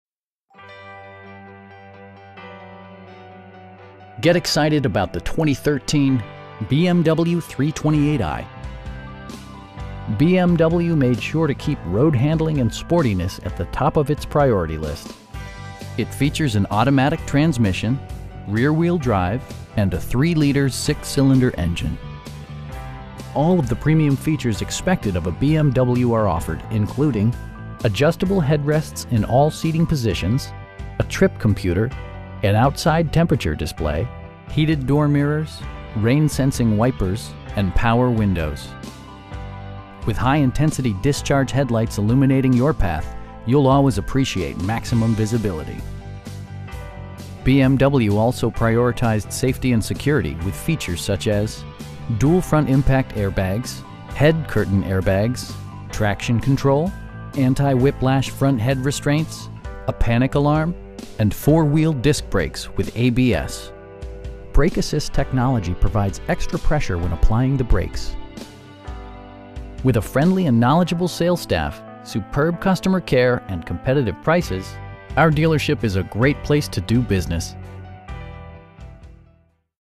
2013 BMW 3 Series 328i in Duluth, GA 30096
Get excited about the 2013 BMW 328i. BMW made sure to keep road-handling and sportiness at the top of it's priority list. It features an automatic transmission, rear-wheel drive, and a 3 liter 6 cylinder engine. All of the premium features expected of a BMW are offered, including: adjustable headrests in all seating positions, a trip computer, an outside temperature display, telescoping steering wheel, heated door mirrors, rain sensing wipers, and power windows. With high intensity discharge headlights illuminating your path, you'll always appreciate maximum visibility. BMW also prioritized safety and security with features such as: dual front impact airbags, head curtain airbags, traction control, anti-whiplash front head restraints, a panic alarm, and 4 wheel disc brakes with ABS. Brake assist technology provides extra pressure when applying the brakes. With a friendly and knowledgeable sales staff, superb customer care, and competitive prices, our dealership is a great place to do business.